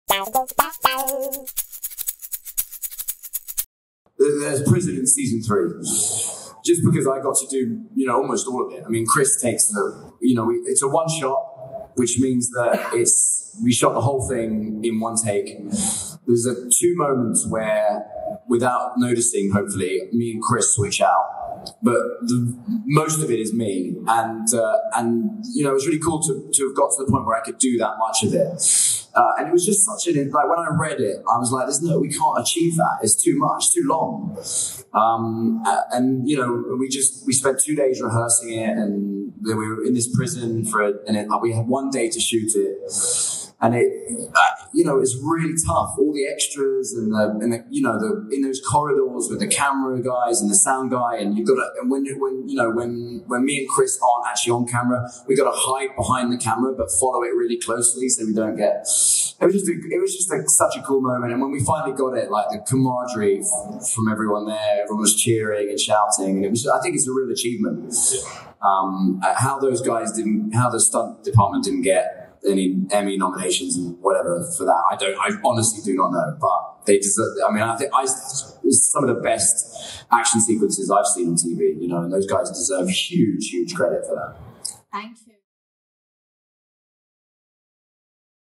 Charlie Cox On The Most Difficult Corridor Fight (Daredevil Panel Clips) - MCM Comic-Con London
Charlie Cox discusses the most difficult of Daredevil's infamous corridor fight sequences to film. Clips from the Daredevil Panel at MCM Comic-Con London in October 2021.